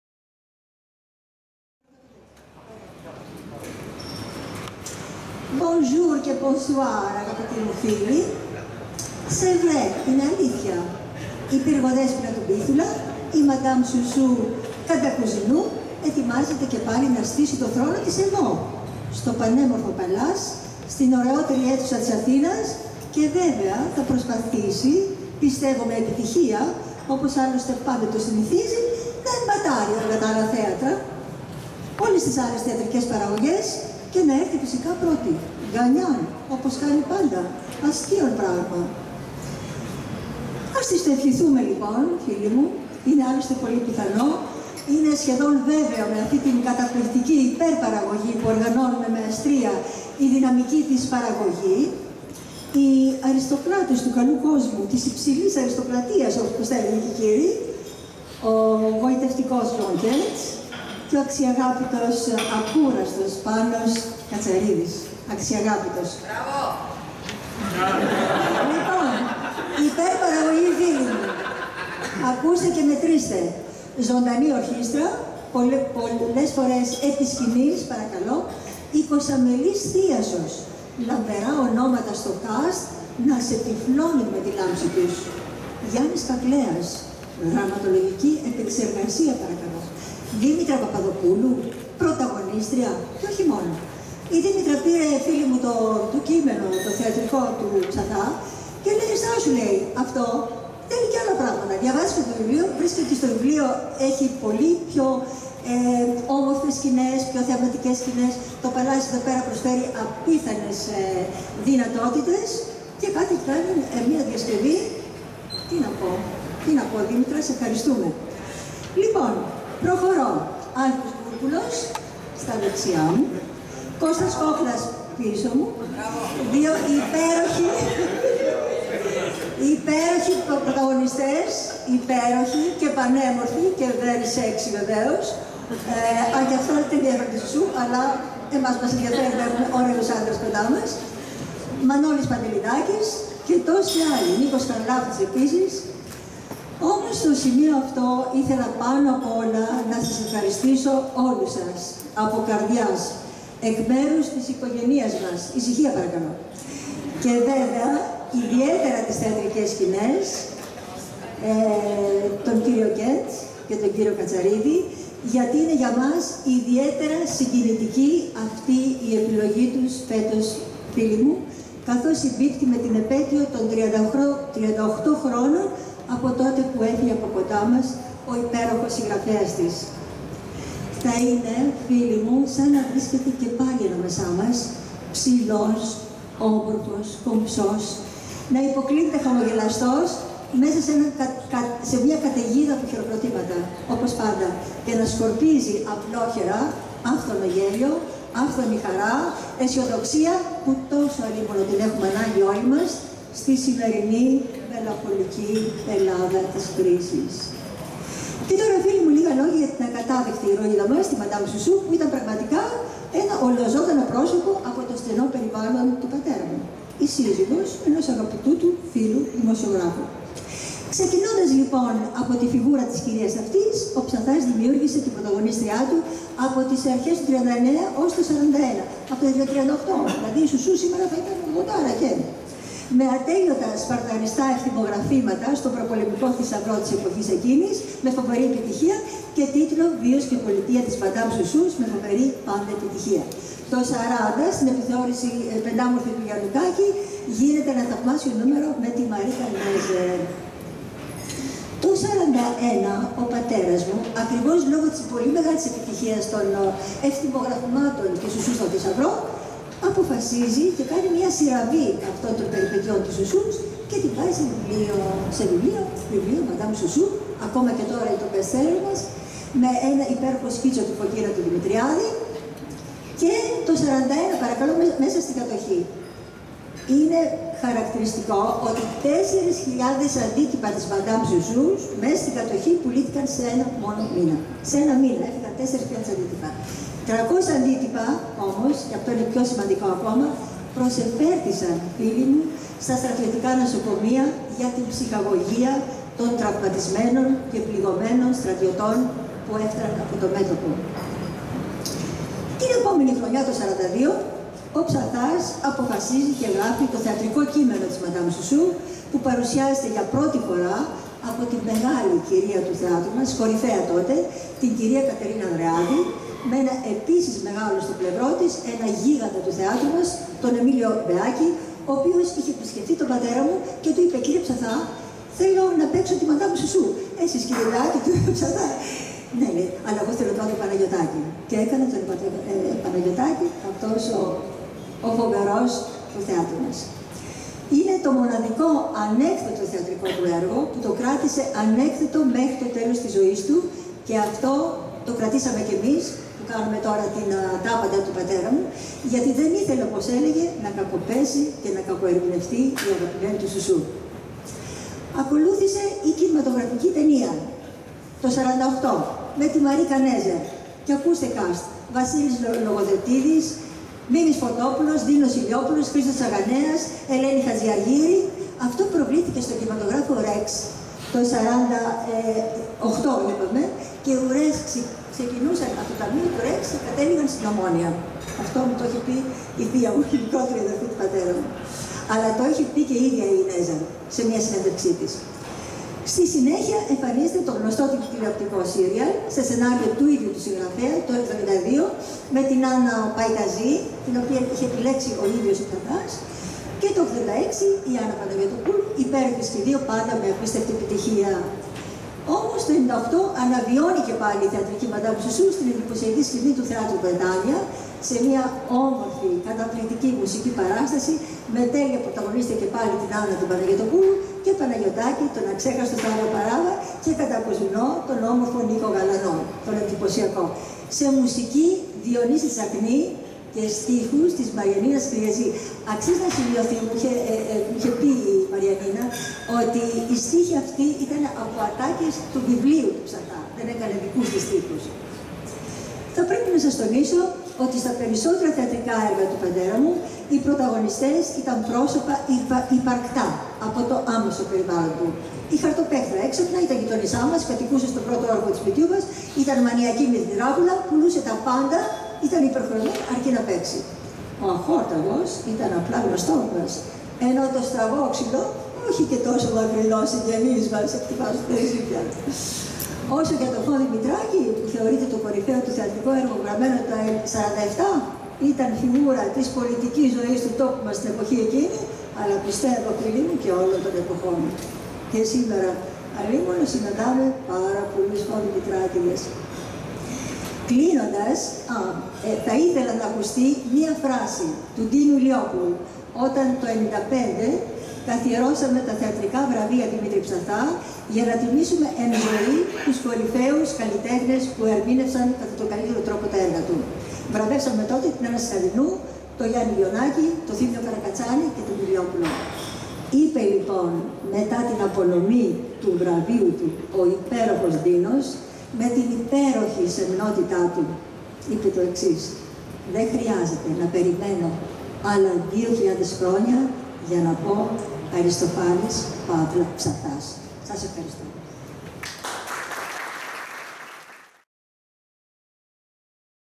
Η Μαρία Ψαθά για τη «Μαντάμ Σουσού»
Η κόρη του Δημήτρη Ψαθά μιλά για την σχεδόν 80ετή πορεία της ονειροπαρμένης ηρωίδας και για τις «περιπέτειες» της στο χαρτί, το σανίδι αλλά και την TV, στη διάρκεια της συνέντευξης Τύπου, στις 28 Σεπτεμβρίου, για τη νέα μεταφορά της «Μαντάμ Σουσού» στο Θέατρο Παλλάς.